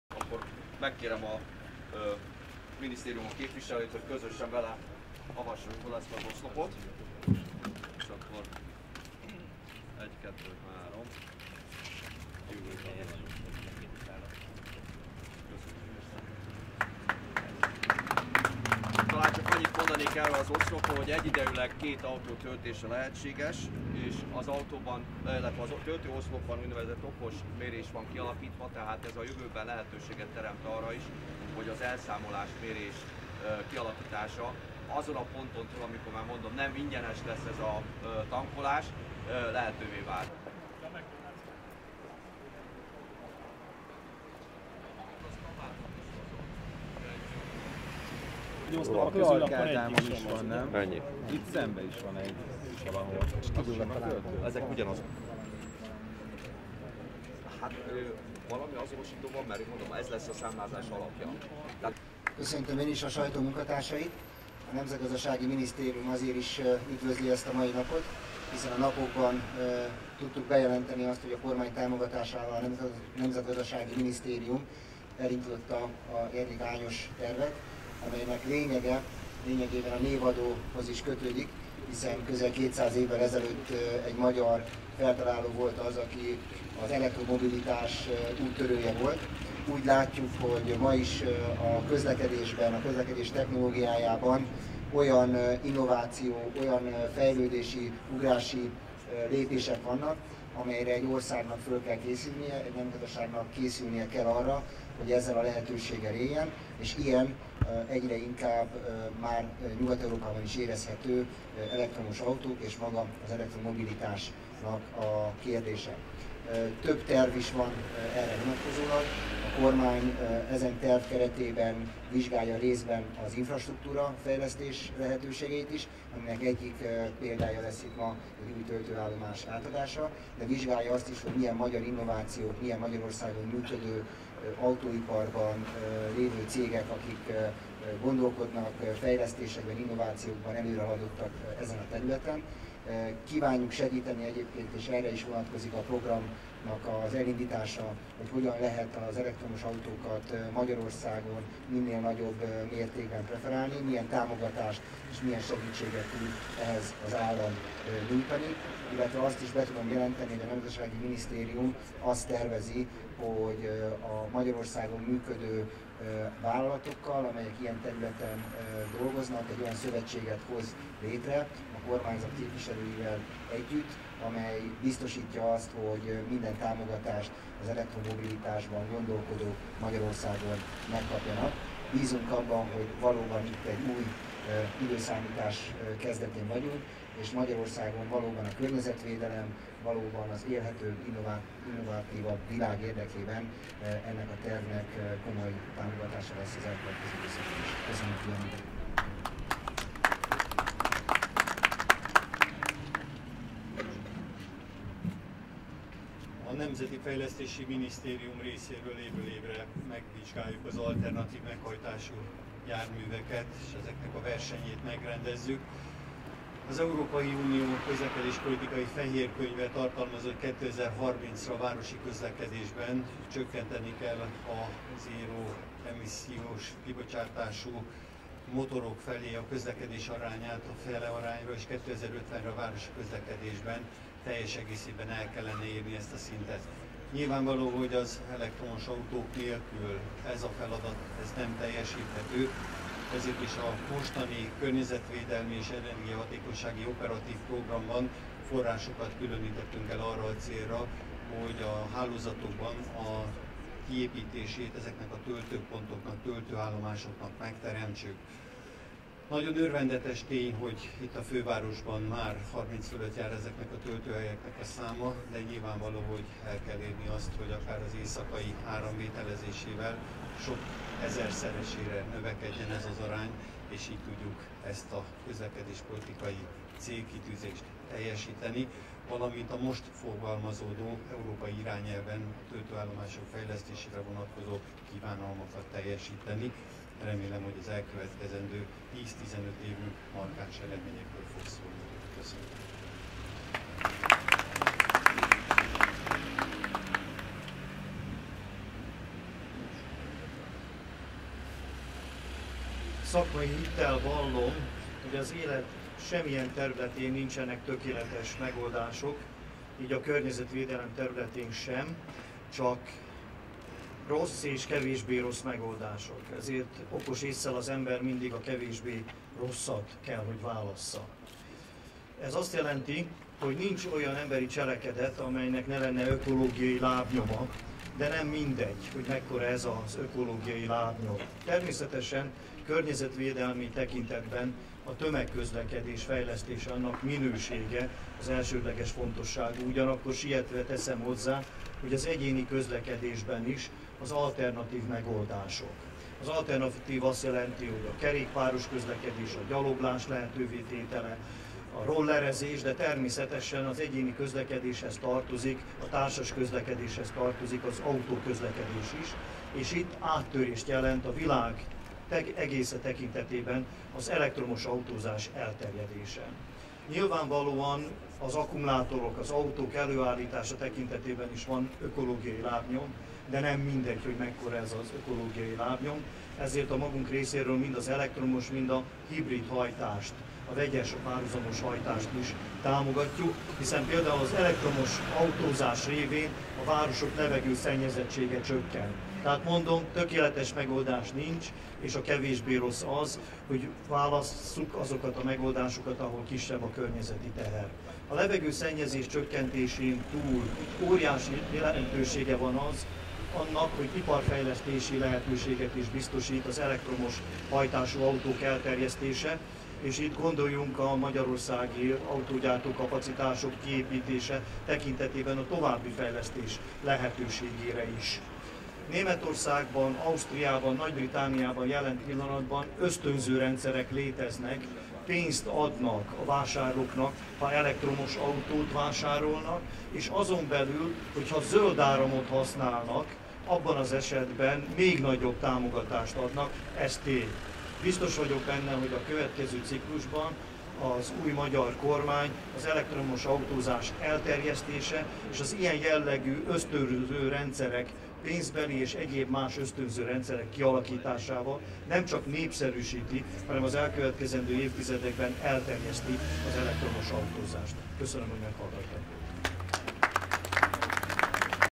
Új elektromos töltőoszlopot adtak át Budapesten
Az előkészítés alatt álló Jedlik Ányos program lényege, hogy az ország felkészüljön a közlekedés technikai innovációjára, így az elektromobilitásra is - mondta Szatmáry Kristóf, a Nemzetgazdasági Minisztérium gazdaságszabályozásért felelős államtitkára egy új elektromos autó töltőoszlop átadásán kedden Budapesten.